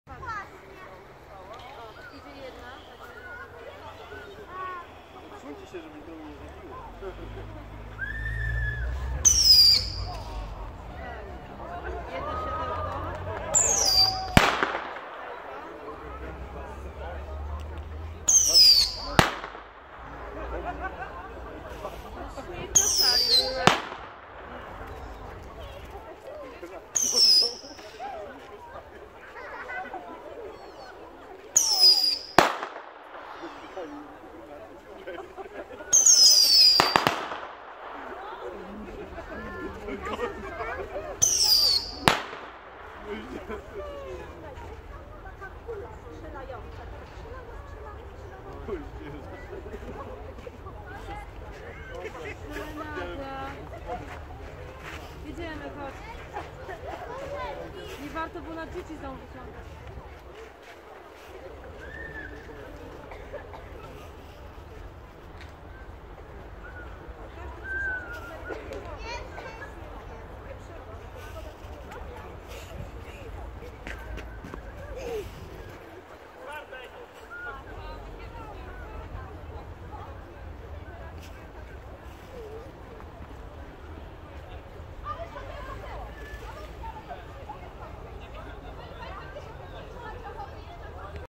Światełko do nieba Augustów 2017 WOŚP.
Augustowskie placówki kultury pokazali na co ich stać.